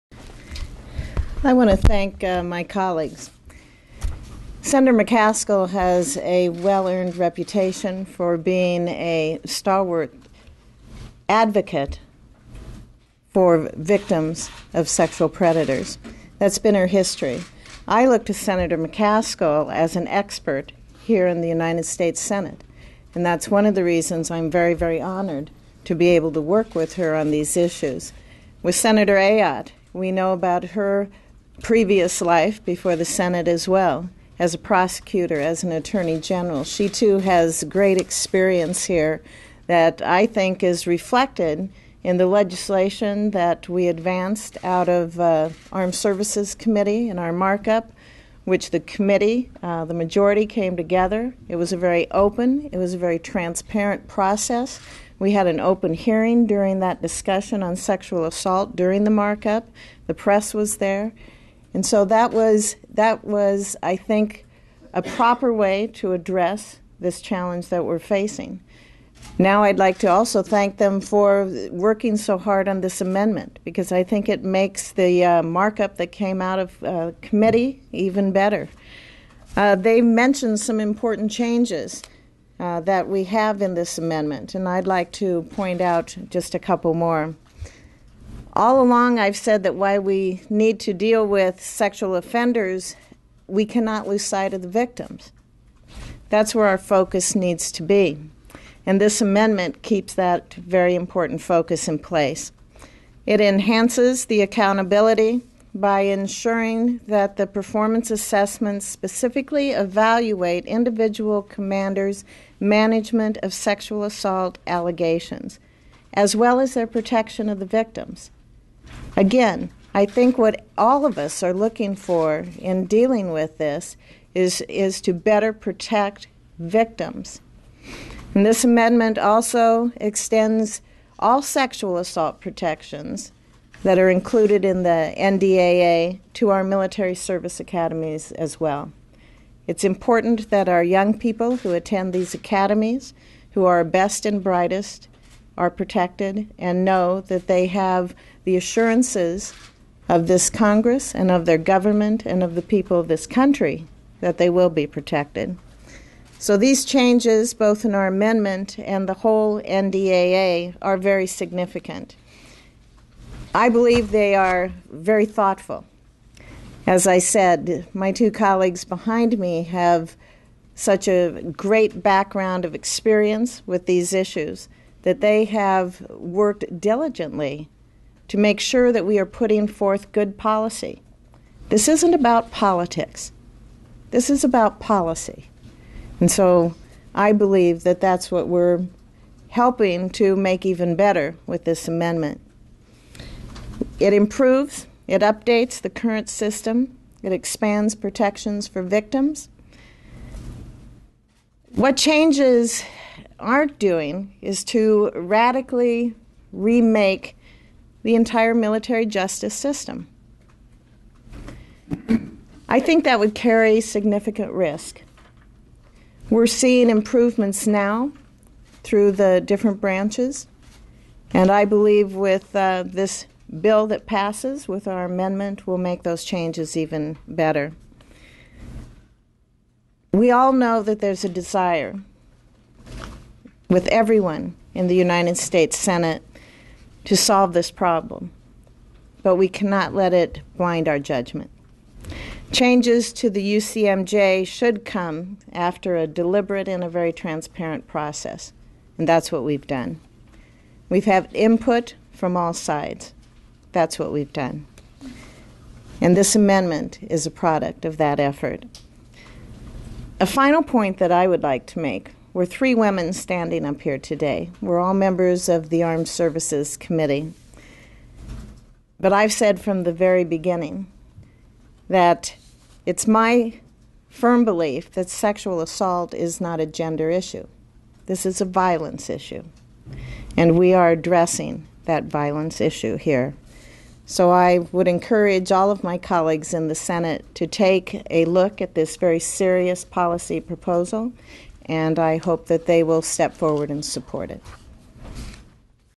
Fischer Introduces Amendment to Protect Military Sexual Assault Victims
U.S. Senator Deb Fischer (R-Neb.) introduces her bipartisan amendment with Senators Claire McCaskill (D-MO) and Kelly Ayotte (R-NH) that will strengthen historic reforms passed by the Senate Armed Services Committee to curb sexual assault in the military.